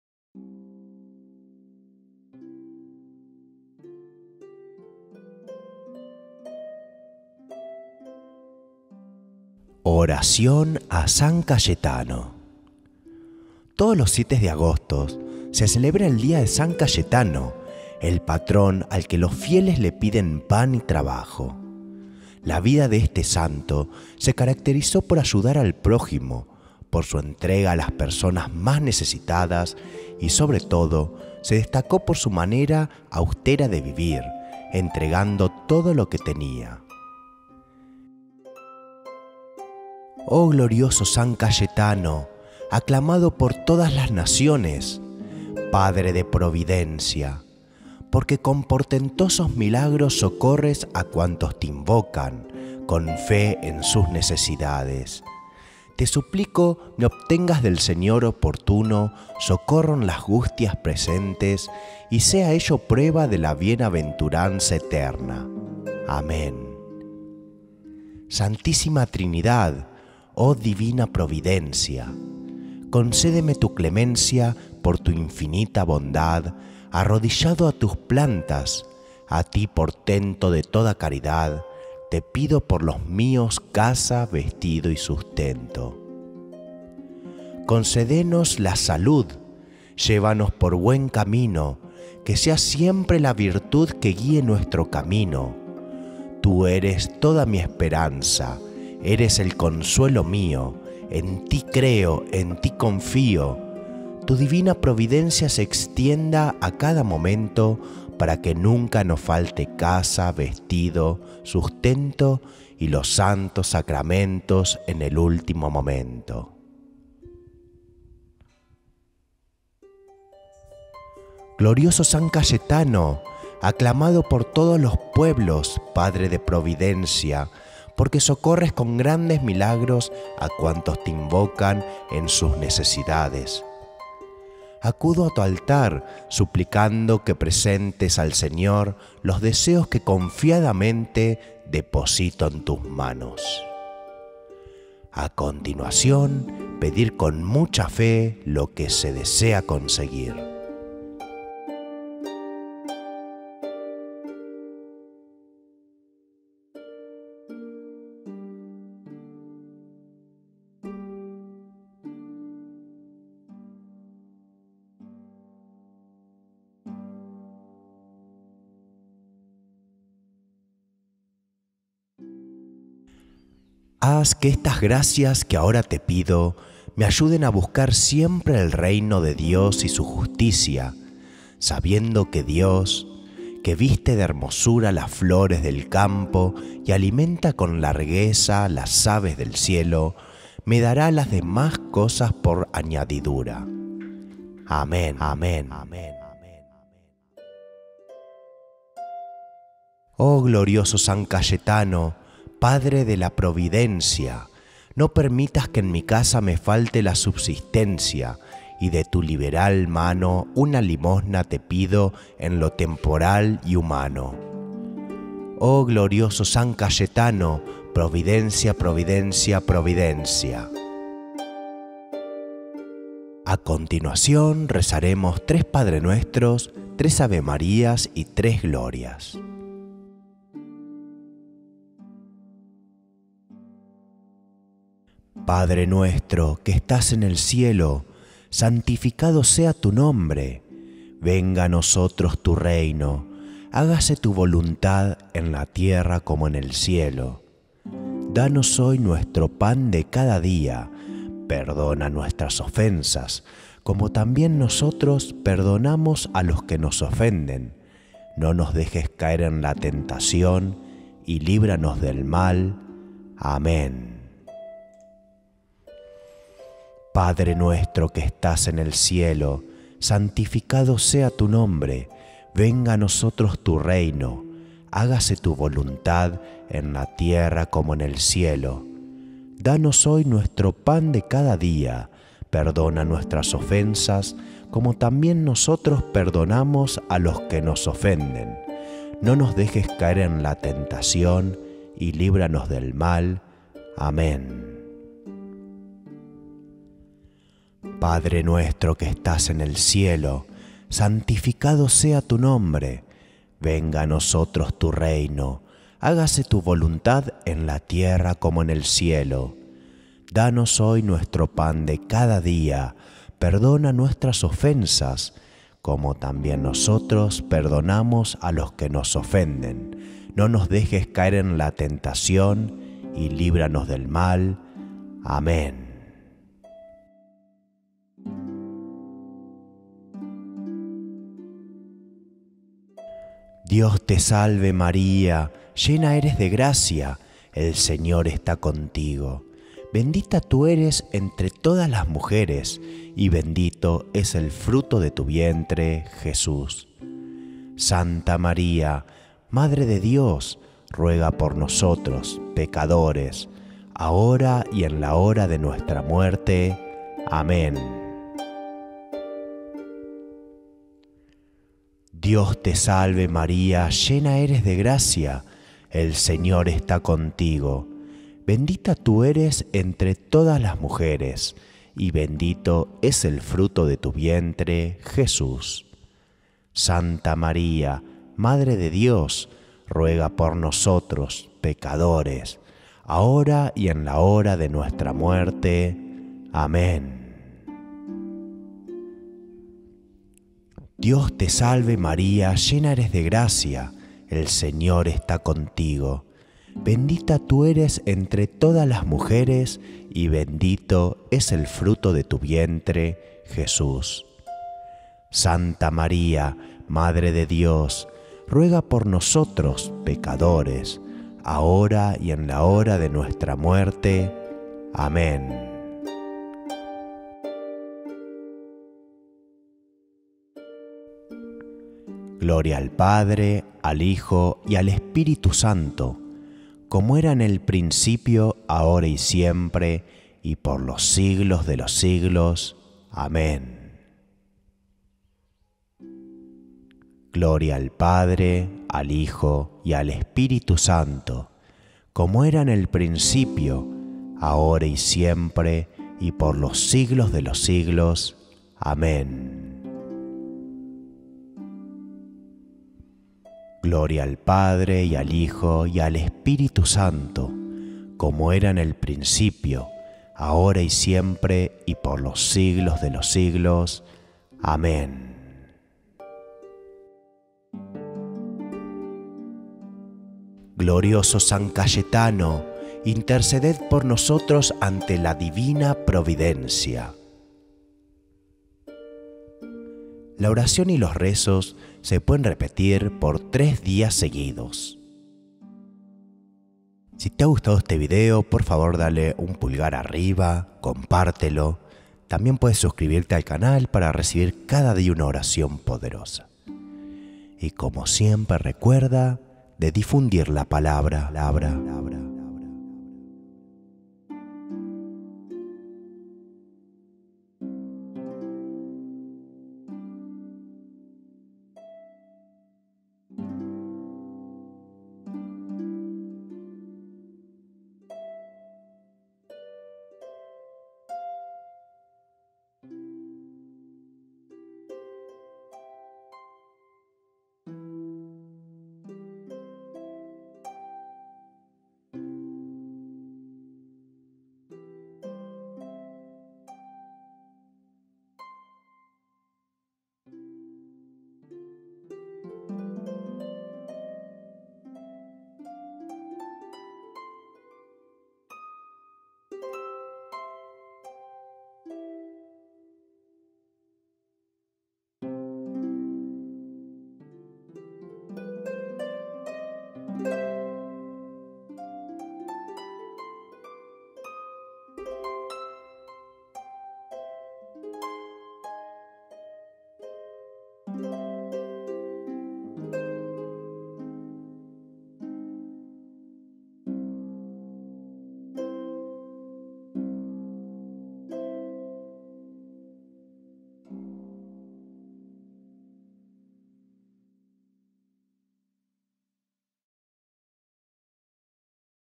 Oración a San Cayetano
- Por favor si te ha gustado el video dale una manito para arriba y compártelo para ayudarme a difundir la palabra.

¡Oh glorioso San Cayetano! Aclamado por todas las Naciones; Padre de Providencia, porque con portentosos milagros socorres a cuantos te invocan con fe en sus necesidades. Te suplico me obtengas del Señor oportuno Socorro en las angustias presentes y sea ello prueba de la bienaventuranza eterna. Amén.

Santísima Trinidad ¡Oh Divina Providencia! Concédeme tu clemencia, por tu infinita bondad, arrodillado a tus plantas, a Ti portento de toda caridad, te pido por los míos casa, vestido y sustento.

Concédenos la salud, llévanos por buen camino, que sea siempre la virtud que guie nuestro destino. Tú eres toda mi esperanza, eres el consuelo mío, en Ti creo, en Ti confío. Tu Divina Providencia se extienda a cada momento para que nunca nos falte casa, vestido, sustento y los Santos Sacramentos en el último momento.

Glorioso San Cayetano, aclamado por todos los pueblos padre de providencia porque socorres con grandes milagros a cuantos te invocan en sus necesidades: acudo a tu altar, suplicando que presentes al Señor los deseos que confiadamente deposito en tus manos.

(Aquí se expresan las gracias que se desea obtener)

Haz que estas gracias, que ahora te pido, me ayuden a buscar siempre el Reino de Dios y su Justicia, sabiendo que Dios (que viste de hermosura las flores del campo y alimenta con largueza las aves del cielo) me dará las demás cosas por añadidura. Amén.

¡Oh glorioso San Cayetano Padre de la Providencia!, no permitas que en mi casa me falte la subsistencia y de tu liberal mano una limosna te pido en lo temporal y humano.

¡Oh glorioso San Cayetano!, Providencia, Providencia, Providencia.